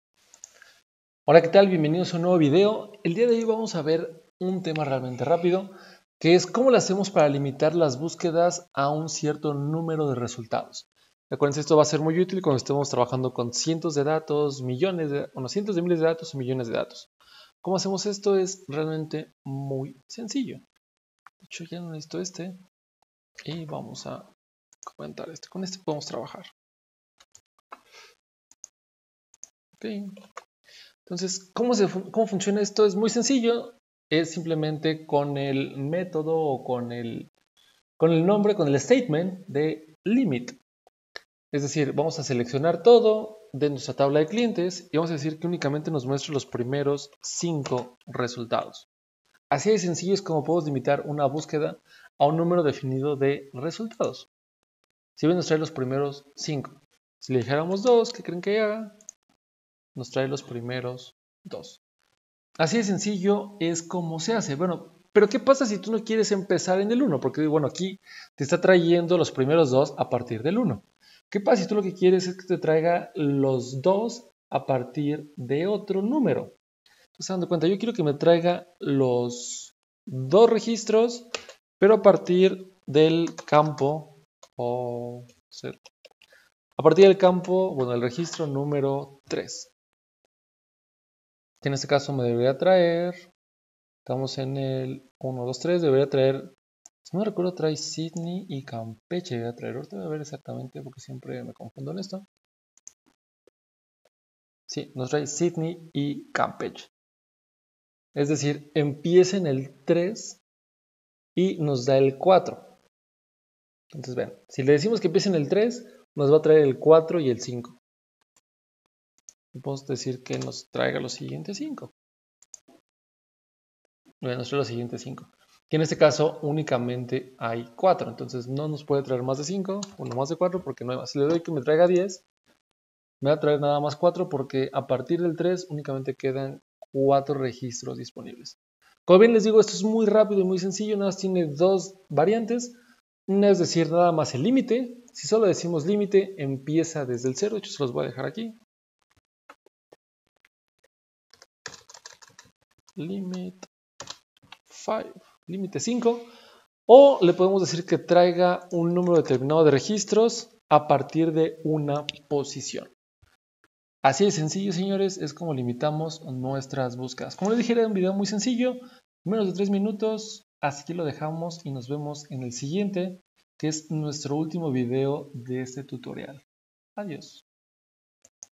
MySQL con Python - V10 - Cómo limitar Búsquedas en MySQL
Curso completo de MySQL y Python

En este curso aprenderás todo lo necesario para trabajar con bases de datos tipo MySQL desde Python